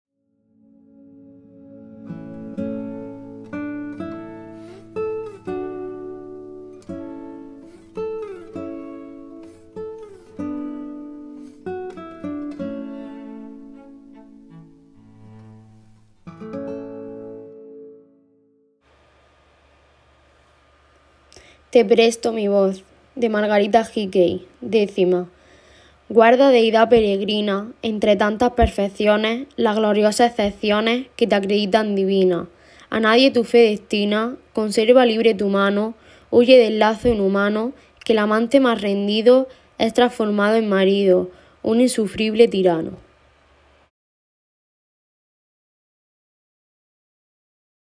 Margarita Hickey
Elena, alumna del IES Alhadra, lee décima de Margarita Hickey.